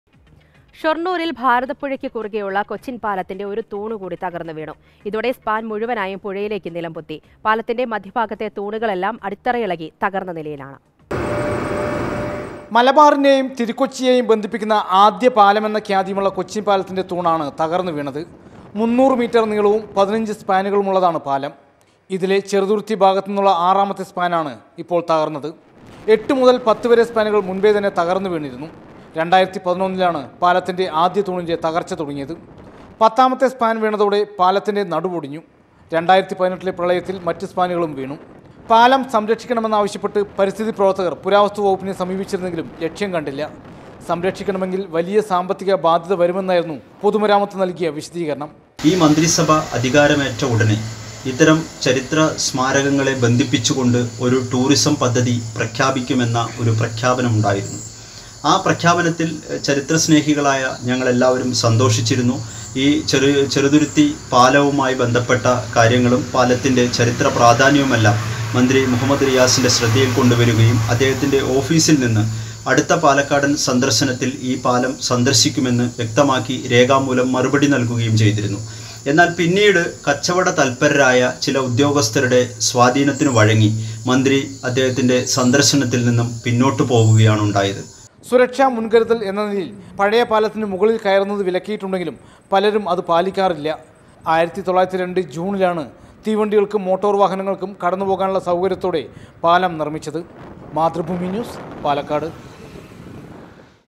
ഭാരതപ്പുഴയ്ക്ക് കുറുകെയുള്ള കൊച്ചിൻ പാലത്തിന്‍റെ ഒരു തൂണ് കൂടി തകർന്നു | Mathrubhumi News
ഷൊർണൂരിൽ ഭാരതപ്പുഴയ്‌ക്ക് കുറുകെയുള്ള കൊച്ചിൻപാലത്തിന്റെ ഒരു തൂണുകൂടി തകർന്നു വീണു. ഇതോടെ സ്പാൻ മുഴുവനായും പുഴയിലേക്ക് നിലംപൊത്തി. പാലത്തിന്റെ മധ്യഭാഗത്തെ തൂണുകളെല്ലാം അടിത്തറയിളകി തകർന്ന നിലയിലാണ്

Happy viewing!